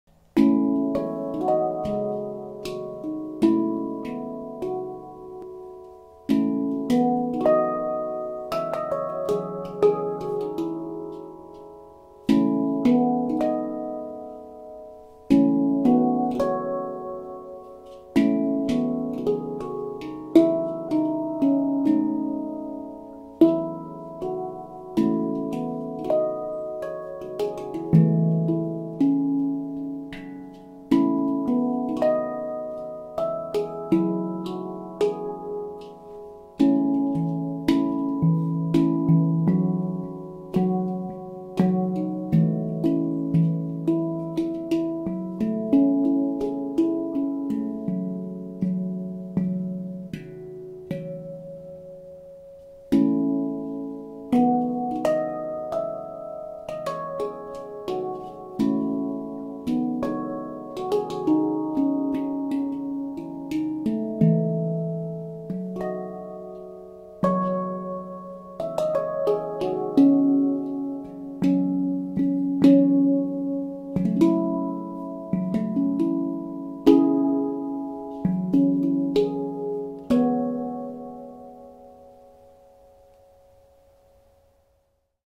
The creating handpan phrase | Improvisation time 23
Challenging myself and creating.
I hope you like what you hear and see.

Pantam : Hoia Pantam
Scale : F# La Sirena 16